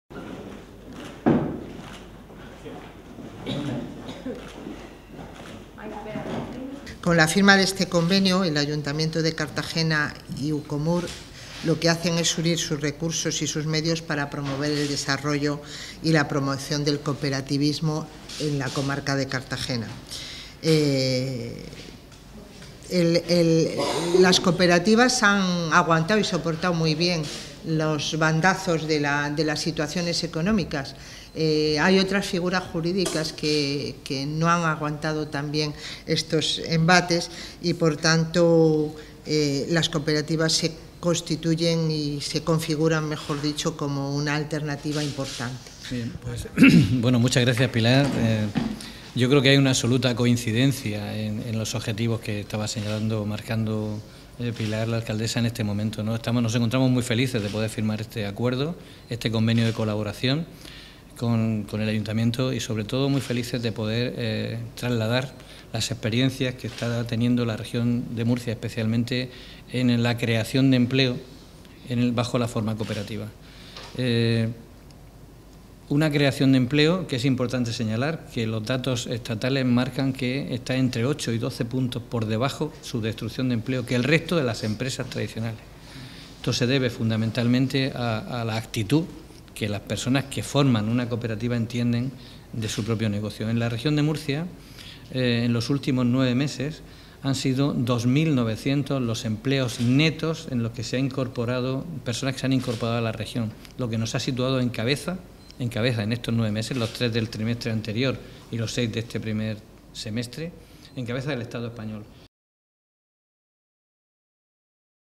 UCOMUR promocionará las cooperativas como alternativa empresarial para crear empleo en Ca
El hecho de que las cooperativas hayan sido el único modelo empresarial que ha aguantado bien los bandazos de la crisis económica, las convierte en objeto del convenio que han firmado esta mañana la alcaldesa, Pilar Barreiro, en nombre del Ayuntamiento, y el presidente de la Unión de Cooperativas de la Región de Murcia (UCOMUR), Juan Antonio Pedreño, encaminado a la promoción del cooperativismo y la economía social en el Municipio de Cartagena.

Para Barreiro, las cooperativas se han constituido en una alternativa importante a tener en cuenta para empresas que ven cerca el desahucio y, por medio de este convenio, nos coordinaremos, a través de la ADLE y su servicio de asesoramiento a emprendedores, para dar a conocer y promocionar este modelo. Y es que el objetivo de los políticos, aseguraba la alcaldesa, es generar empleo.

Por su parte, Pedreño ha resaltado las cifras alentadoras con respecto a la creación de puestos de trabajo de las cooperativas. Este modelo empresarial ha generado 2.900 empleos en la Región en los últimos nueve meses, lo cual nos coloca a la cabeza de creación de empleo en el país. Además, muchas empresas abocadas al cierre se han reconvertido en cooperativas salvando así sus empleos. En la región hay unas 1.500 cooperativas.

Ahora la asignatura pendiente es Cartagena. Según el presidente de UCOMUR, el municipio necesita un impulso, ya que no figura entre los 10 primeros de la Región en creación de cooperativas. De ahí la necesidad de este convenio, por medio del cual se pondrán en marcha actividades para informar sobre el modelo empresarial, se asesorará a emprendedores y se estudiará la viabilidad de proyectos.